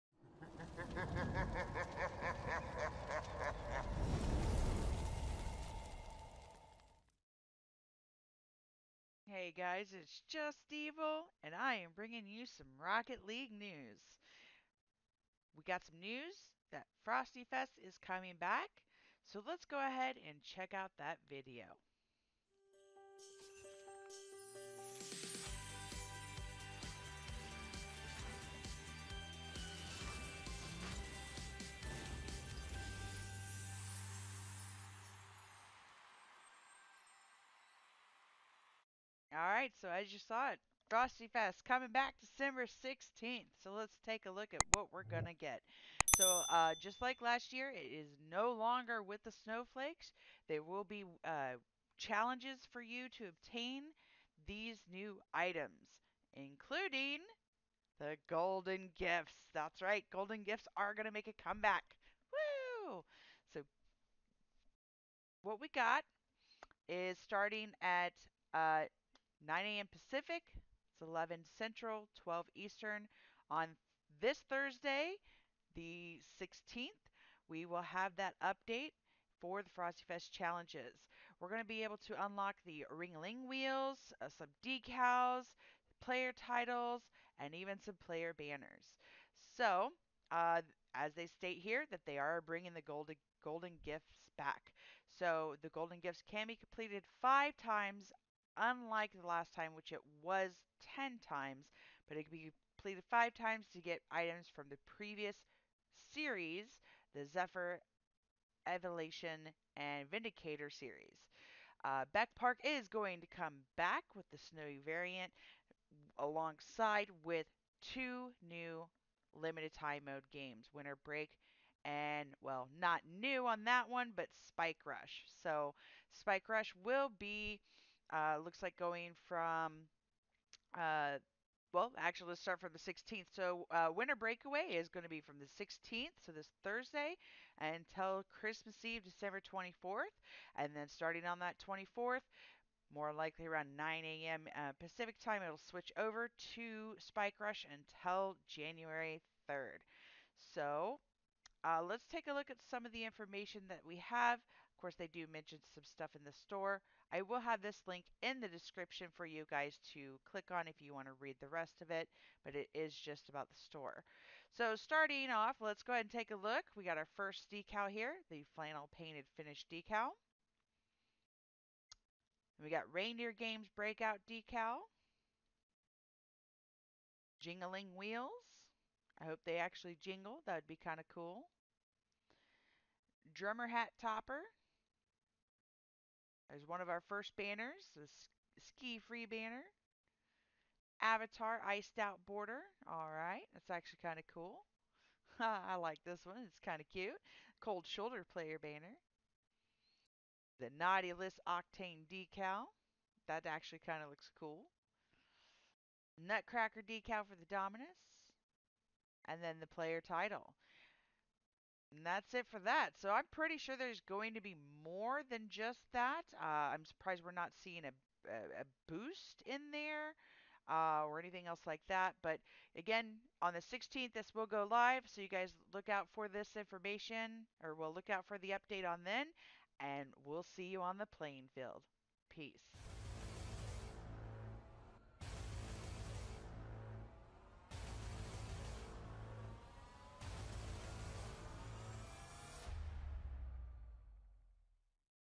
Frost Fest is back! Frost Fest 2021
Frosty Fest is back and packed with all-new Event Challenges and more starting Thursday, December 16th.